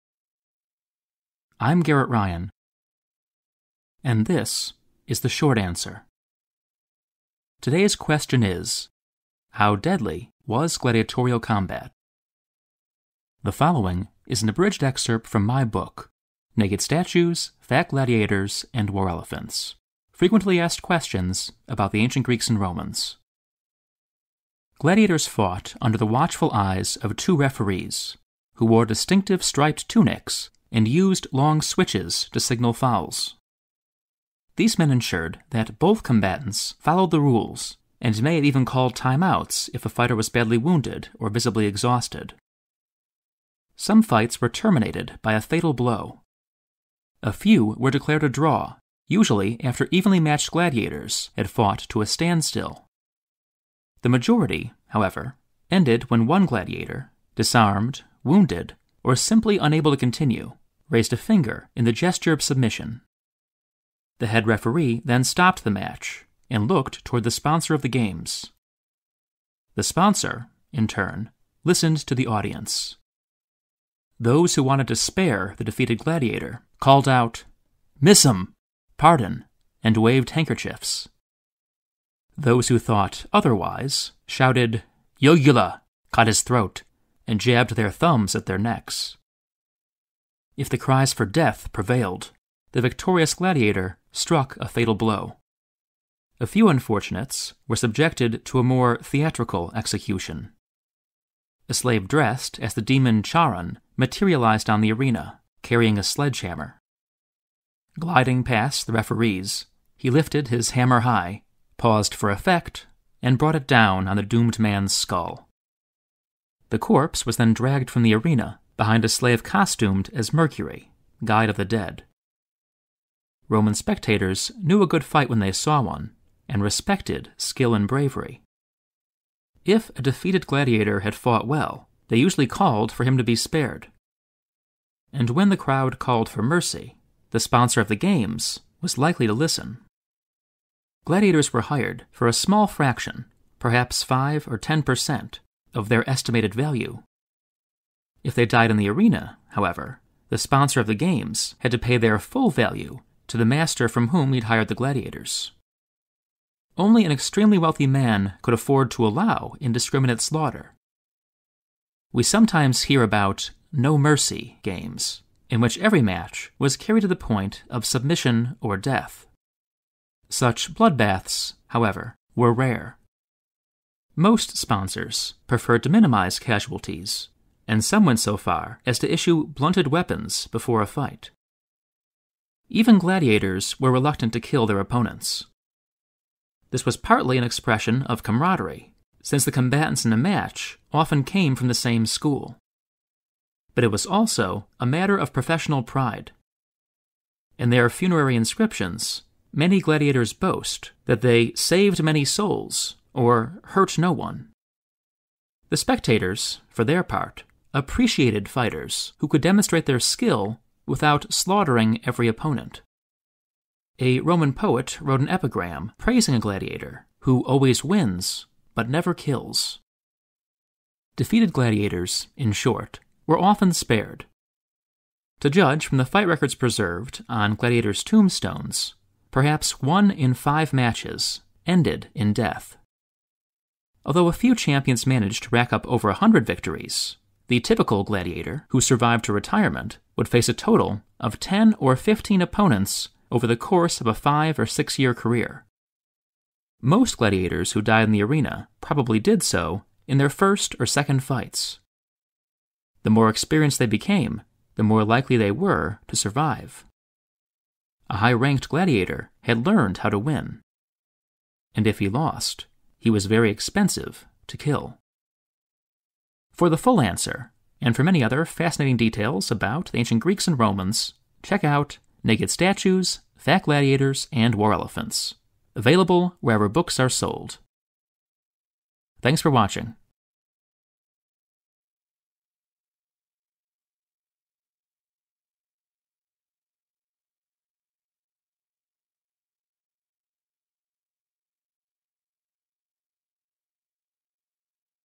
How deadly was gladiatorial combat?
Some gladiatorial matches were terminated by a fatal blow. A few were declared a draw. The majority, however, ended when one of the gladiators raised a finger in the gesture of submission. His fate then depended on the mood of the audience - and the wealth of the games’ sponsor.

For more on the gladiators and many other aspects of classical life, check out my book “Naked Statues, Fat Gladiators, and War Elephants: Frequently Asked Questions about the Ancient Greeks and Romans.”

Chapters:
0:00 Introduction
0:24 Rules and referees
1:09 Death in the arena
2:03 Counting costs
3:02 Merciful gladiators
3:41 The odds of survival
4:26 For more, check out the book